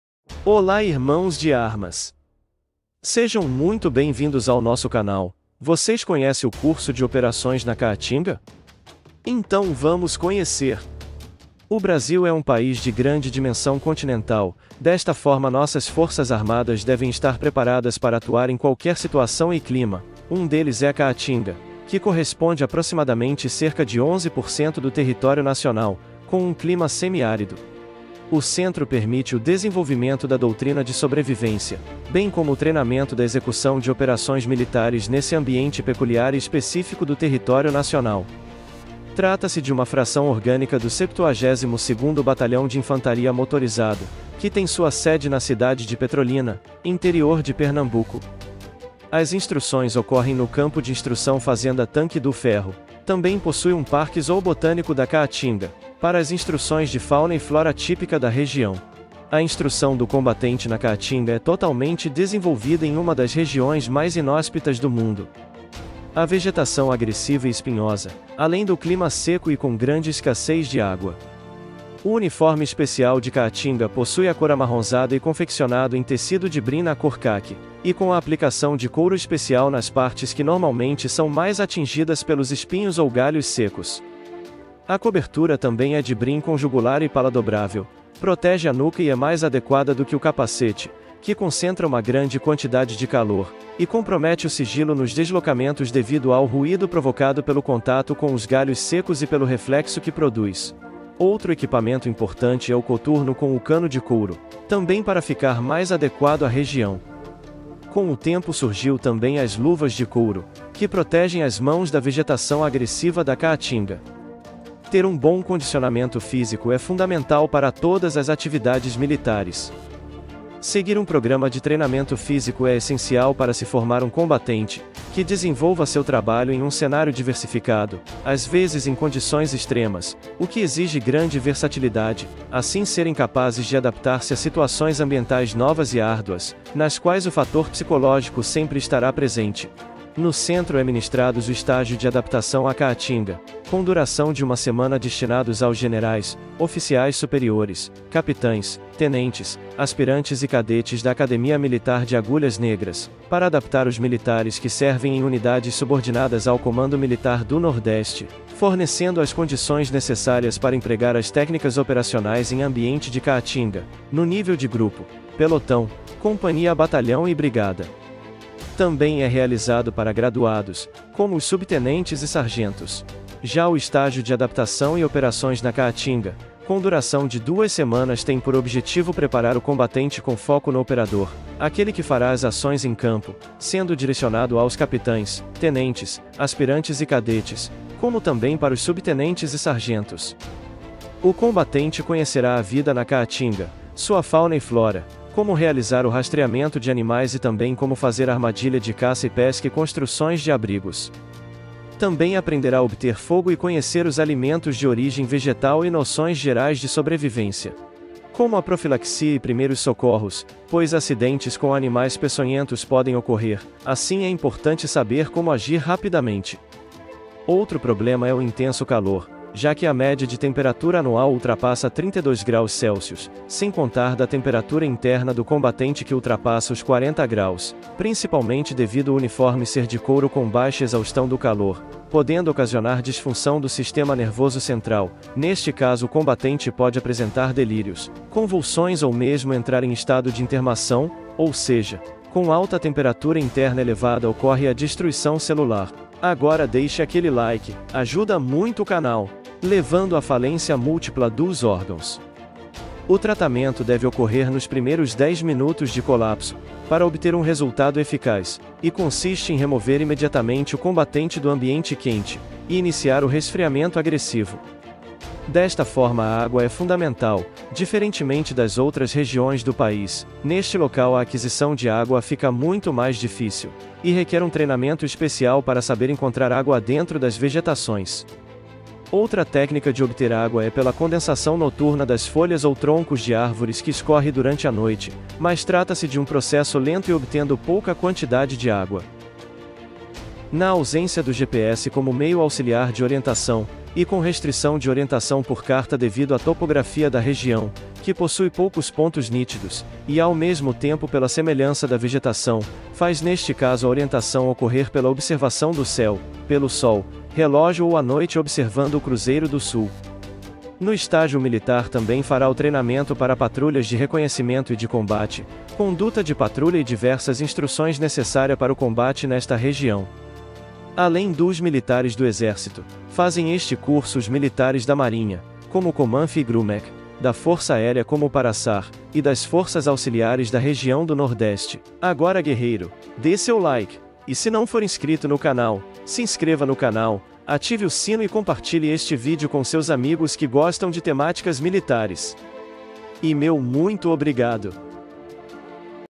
Você comeria olho de bode? Conheça os combatentes do Centro de Instrução de Operações na Caatinga!
O Centro de Instrução e Operações na Caatinga (CIOpC) é, atualmente, o estabelecimento destinado à formação do Combatente de Caatinga do Exército Brasileiro. Trata-se de uma fração orgânica do 72º Batalhão de Infantaria Motorizado (72º BI Mtz), Batalhão General Victorino Carneiro Monteiro, que tem sua sede na cidade de Petrolina, interior de Pernambuco. O CIOpC possui um campo de instrução para as atividades de formação do combatente de caatinga e o Parque Zoobotânico da Caatinga, que é o seu apoio para as instruções de fauna e flora.
Nesse Centro, são ministrados o Estágio de Adaptação à Caatinga, com duração de uma semana, e o Estágio de Adaptação e Operações na Caatinga, com duração de duas semanas.
As atividades de instrução desenvolvem-se em ritmo intenso, buscando reproduzir as condições de combate em área de caatinga. Em meio à sequidão do sertão, encontra-se o Campo de Instrução Fazenda Tanque do Ferro (CIFTF), com uma área de 2.800 hectares, totalmente inserido em uma região coberta pelo bioma caatinga e afastado em relação aos grandes centros urbanos. Tais características fazem dele um local perfeito para o desenvolvimento de pesquisa e validação da doutrina a respeito das operações militares nesse tipo de ambiente.
A instrução do combatente na caatinga é totalmente desenvolvida em uma das regiões mais inóspitas do mundo. A vegetação agressiva e espinhosa; o clima causticaste; o relevo modesto e ondulado; solos erodidos e muitas vezes pedregosos; a paisagem uniforme com aglomerados humanos esparsos e uma grande escassez de água. Tudo exige do homem um treinamento especial para suplantar tais dificuldades.
O uniforme do combatente da caatinga é diferente com aplicações em couro de cor amarronzada, lembrando os trajes dos vaqueiros da região, confunde o homem com o meio ambiente. A cobertura é dotada de abas, protegendo o rosto e a nuca do sol. Tanto a calça quanto a camisa apresentam reforços de couro no peito, nos braços e nas pernas, para melhor suportar a agressão da vegetação e do solo.

O 72º Batalhão de Infantaria Motorizado foi criado por transformação da 2ª Companhia de Fuzileiros do 35° Batalhão de Infantaria, de Feira de Santana (BA), e iniciou oficialmente suas atividades em 1975. Integrante da 10ª Brigada de Infantaria Motorizada, possui uma área de responsabilidade que abrange, além dos municípios de Petrolina (PE) e Juazeiro (BA), mais de 38 municípios circunvizinhos, sendo 36 no sertão pernambucano e dois no sertão baiano.
A caatinga está, principalmente, no sertão do Brasil e existe a ideia de que o sertanejo é, antes de tudo, forte e resistente, em face das agruras do semiárido. Isso levou à designação, em 1982, do 72° BI Mtz como o responsável pela condução de pesquisas, estudos e exercícios operacionais em área de caatinga, para a aquisição de conhecimentos e o desenvolvimento da doutrina de emprego da tropa em operações nesse peculiar e árido ambiente. Assim, o 72° BI Mtz é o responsável pela formação do combatente de caatinga.
Com tudo o que foi explicitado, salienta-se que o 72° BI Mtz é presença marcante e positiva do Exército Brasileiro na região, identificando-se com a garra e a força combativa da gente do Nordeste Brasileiro.

O clima semiárido está presente no Brasil na Região Nordeste e no Sudeste (norte de Minas Gerais). Está associado ao importante bioma da caatinga, rico em biodiversidade, endemismos e bastante heterogênea. A área do semiárido corresponde a aproximadamente 982.563,3 km2 (cerca de 11% do território brasileiro). Na classificação brasileira, o clima semiárido é aquele com precipitação inferior a 1.000 mm.
Fonte: Exército Brasileiro